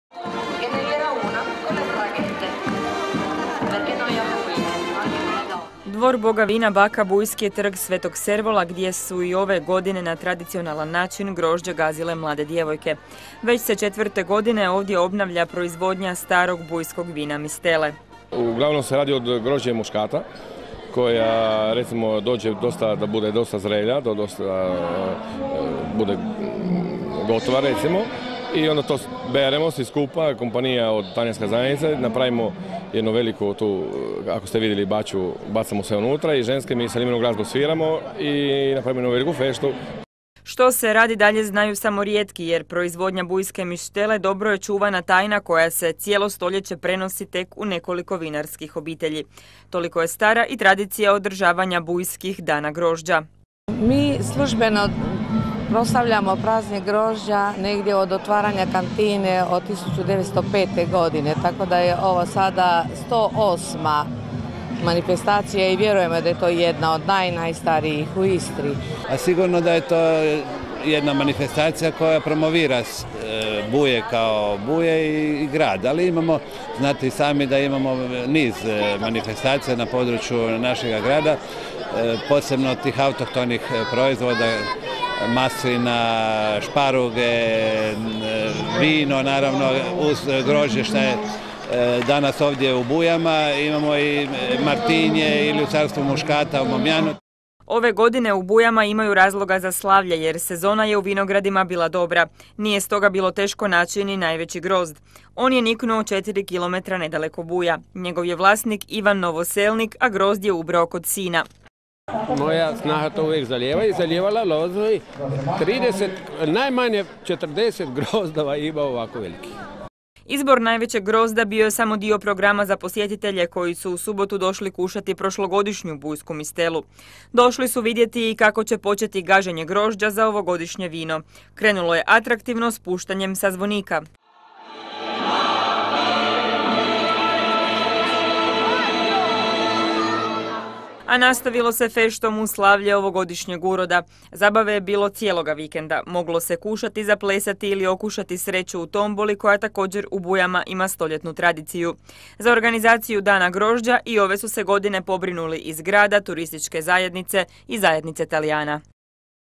U Bujama slavili Dane grožđa
U Bujama su prošloga vikenda slavili Dane grožđa. Gradom je jučer prošla tradicionalna povorka alegorijskih kola, a u subotu je na Bakhovu dvoru počela priprema još jedne bujske mistele.